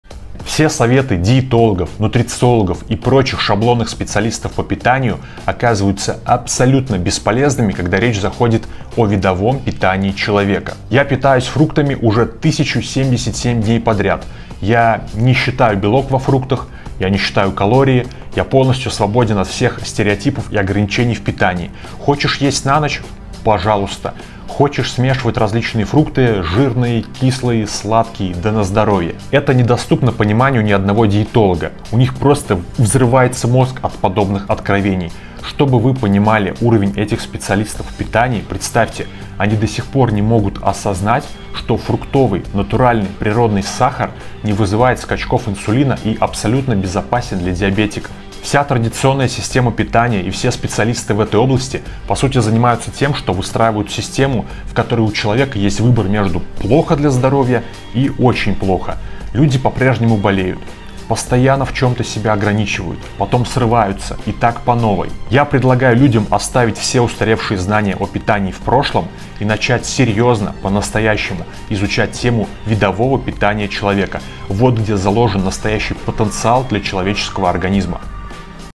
ПРО СОВЕТЫ «СПЕЦИАЛИСТОВ»❗️❗️❗️
❗️НАСТОЯЩАЯ СВОБОДА в питании - нет нужды считать калории, белок, количество микроэлементов… + самый лучший вкус, который не убивает тебя!